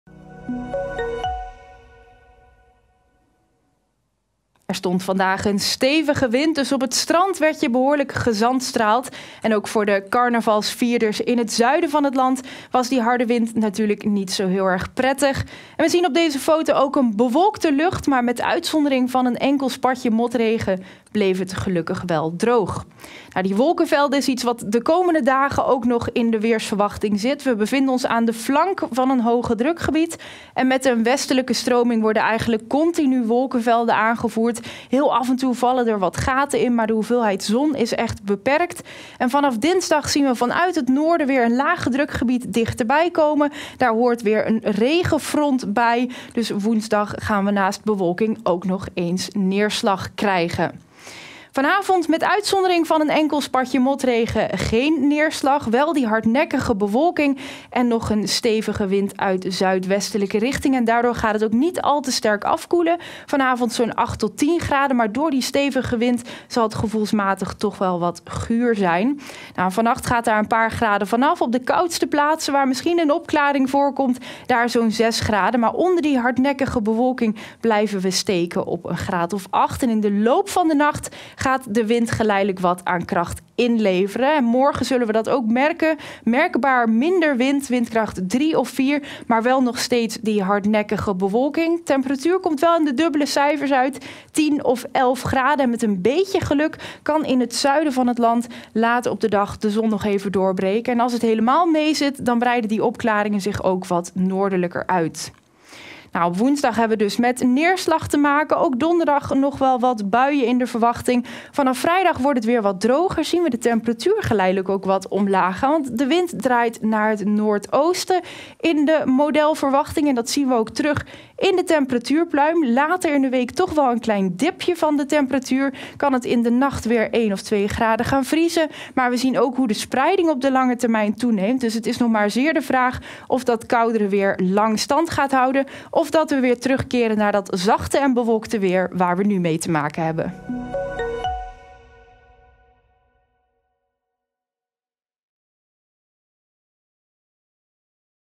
Weerbericht: breekt de zon nog door?
Wolkenvelden passeren ons land en laten daarbij weinig ruimte voor de zon over. Maken opklaringen nog een kans of blijft het bewolkt? Roosmarijn bekijkt het weer van de komende dagen.